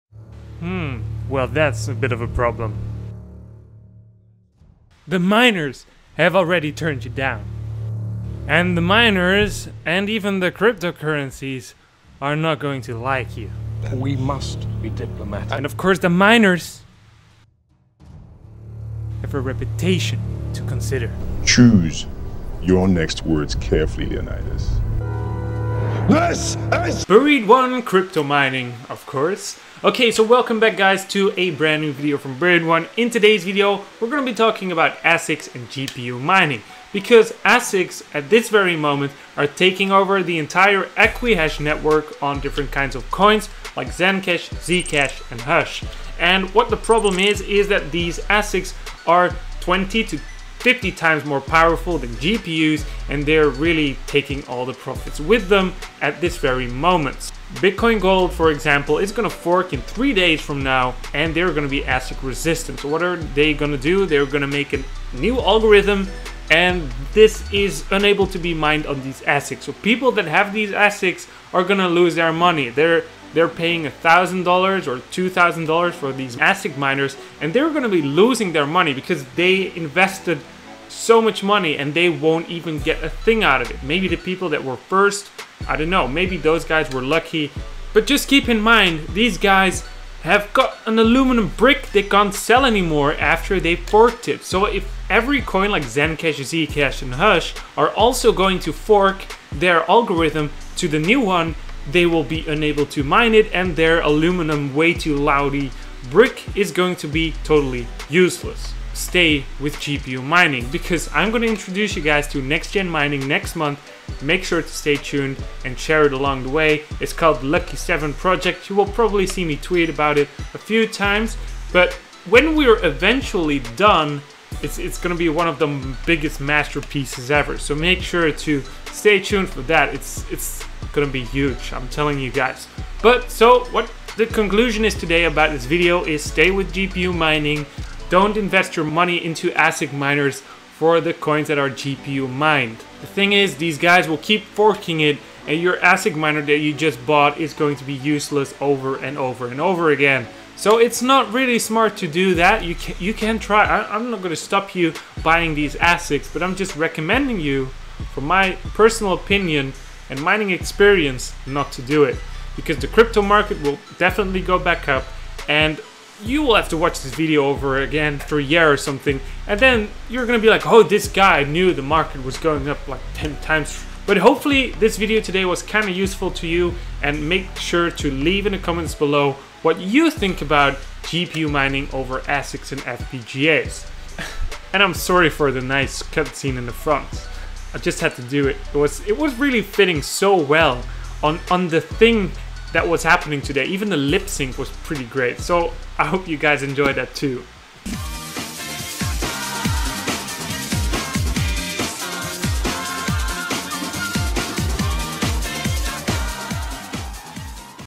Are ASIC Miners Taking Over Equihash GPU Mining?
We can clearly see that ASIC's on the Equihash coins are taking away the current profits for GPU Mining rigs mining these coins.
As we know for now, Bitcoin Gold [BTG] is going to be the first to fork to a new PoW Algorithm for asic resistance on July 1st 2018.
Now it won't take long before all other Equihash based coins will fork against this threat and will turn the ASIC's into noisy aluminum decoration.